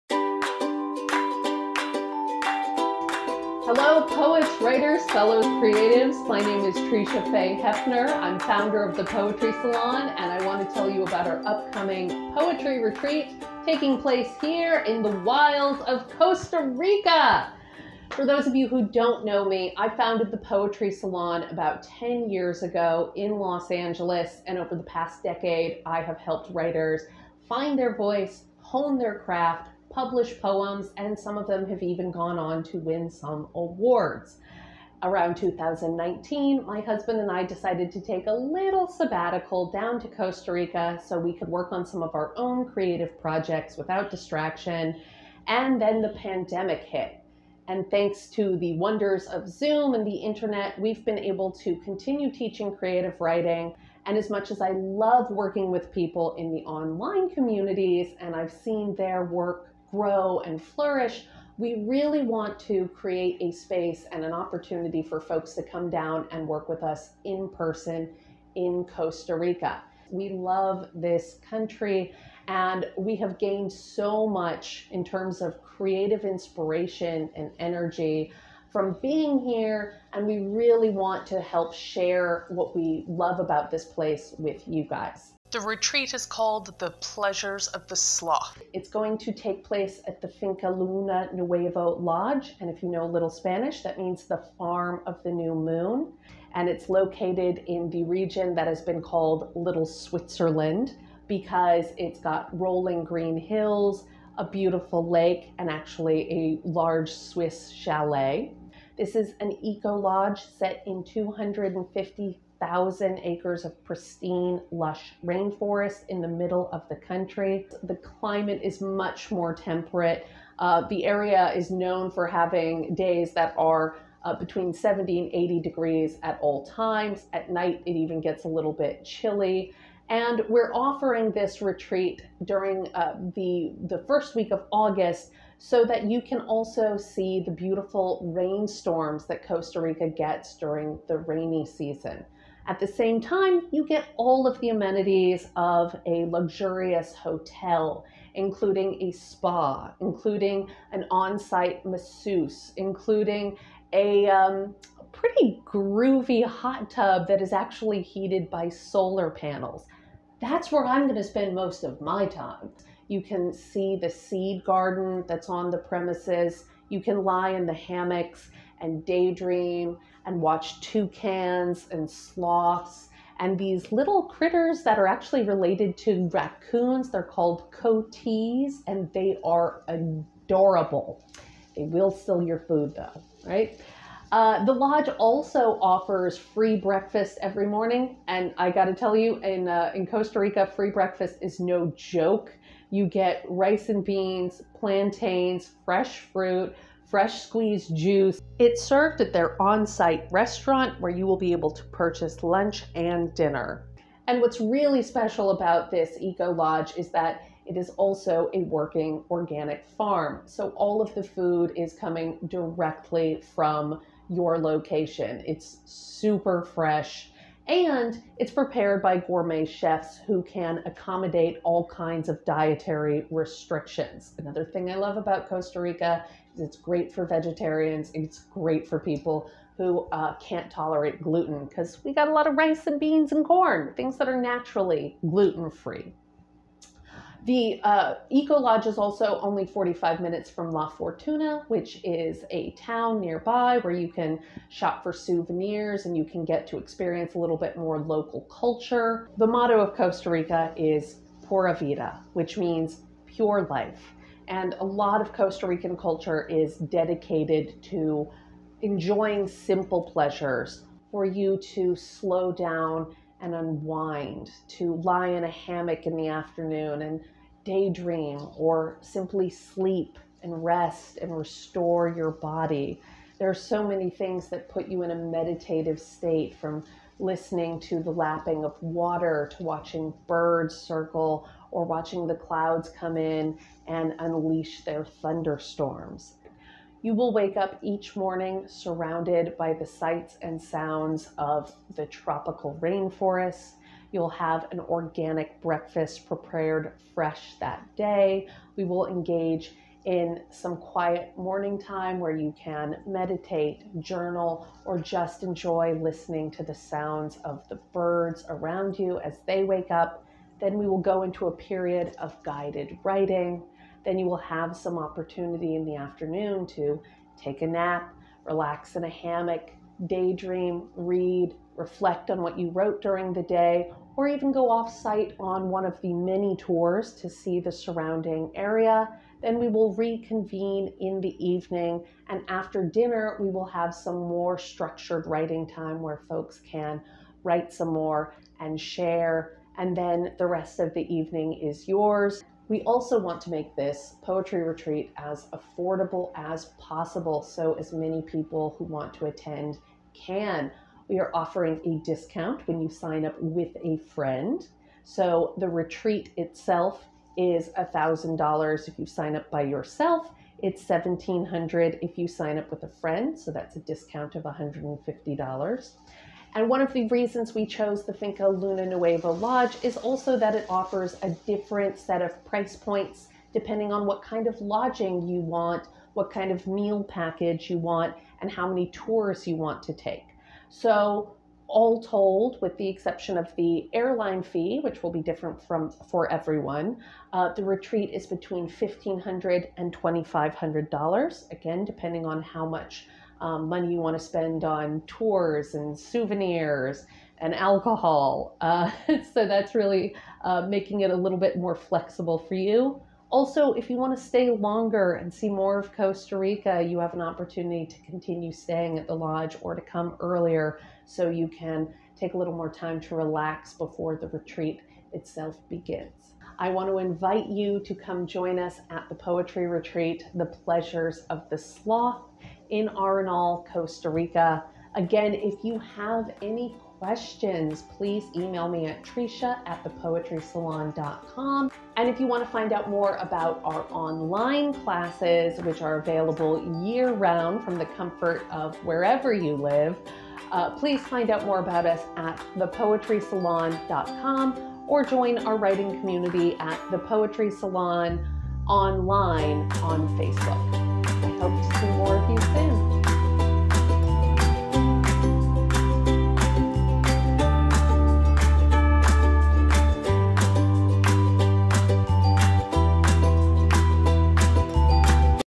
Join the Pleasures of the Sloth Costa Rica Poetry Retreat, July 30th - Aug 6th with The Poetry Salon
The Poetry Salon is hosting our first international retreat in the tropical paradise of Arenal, Costa Rica. 10-writer limit and the cost is $1,000, not including meals, travel and lodging. If you sign up with a friend, the two of you pay only $1,700. The retreat will take place at the gorgeous Finca Luna Nueva EcoLodge. Come relax, expand and write in the beauty of the rainforest.